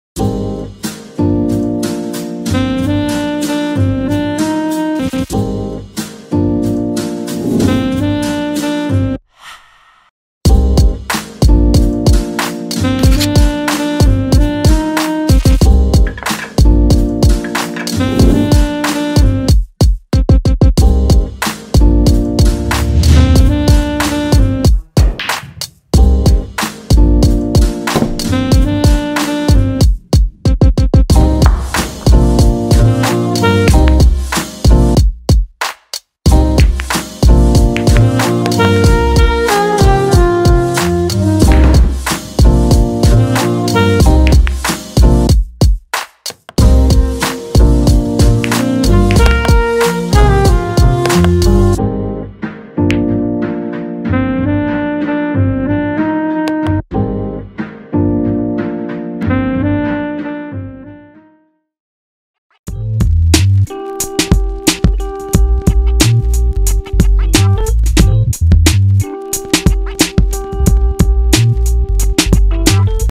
Run the table | Cinematic BROLL
Needed to sell this pool table so we filmed a Cinematic BROLL : p